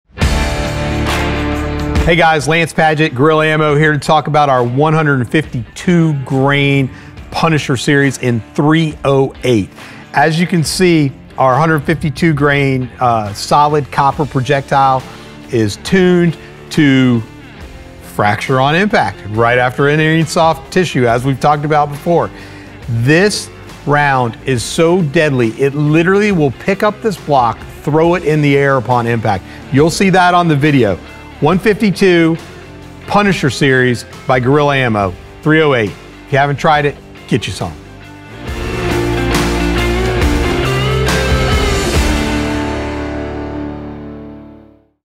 Gorilla Ammunition .308 WIN 152gr Punisher Series - Fracturing
Gorilla's CEO, Lanse Padgett's product review and highspeed video of Gorilla Ammunition's 308 Winchester, 152gr Punisher Series. These solid copper projectiles are turned in house and deliver devastating terminal performance. Great for hunting and self defense.